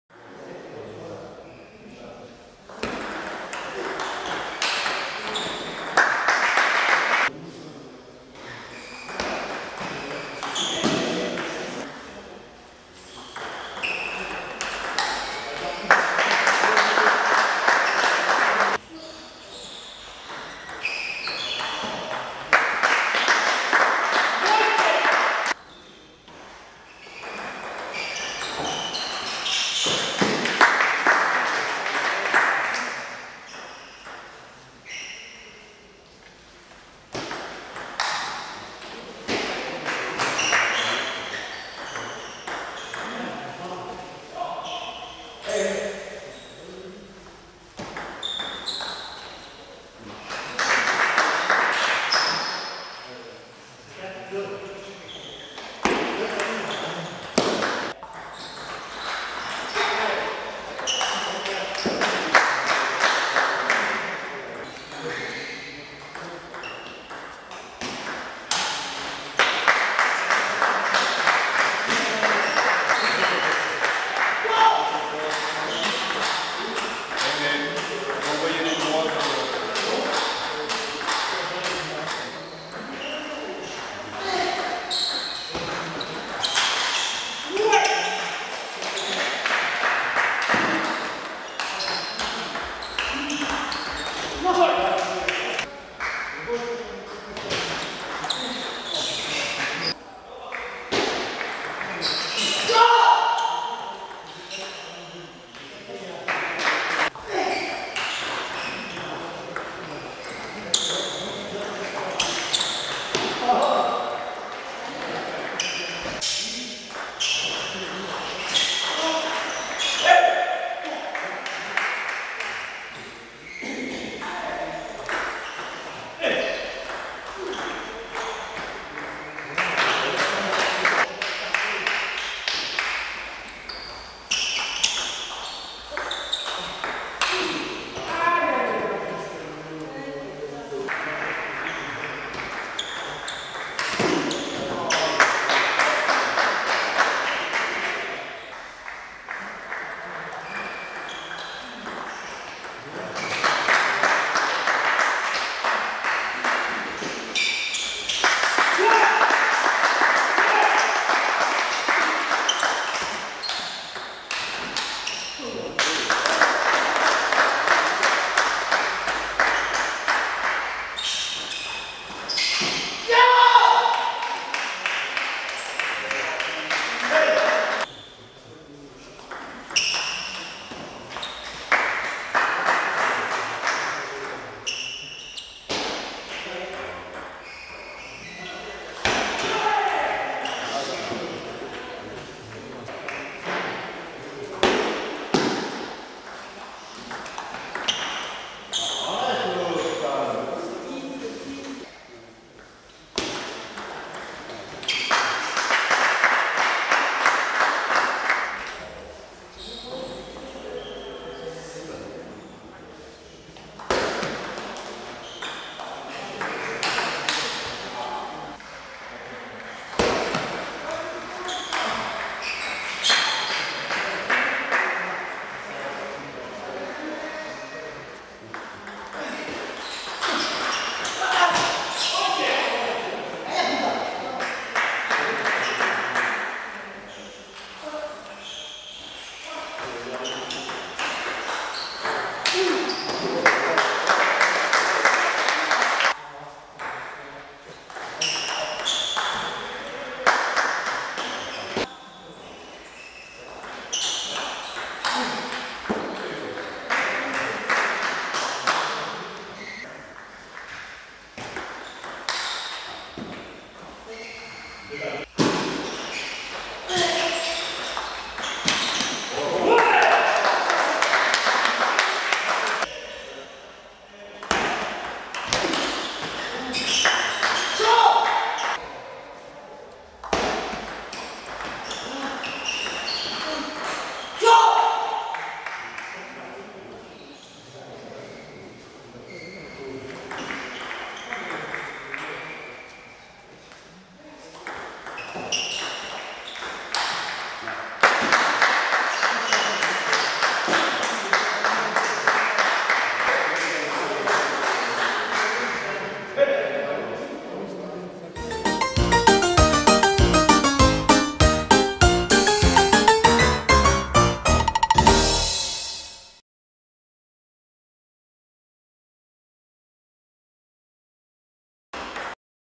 AUVR TT / NICE CAVIGAL 3 (2021)
Extraits doubles  match du championnat de France N3 de tennis de table à Rillieux le 25/9/2021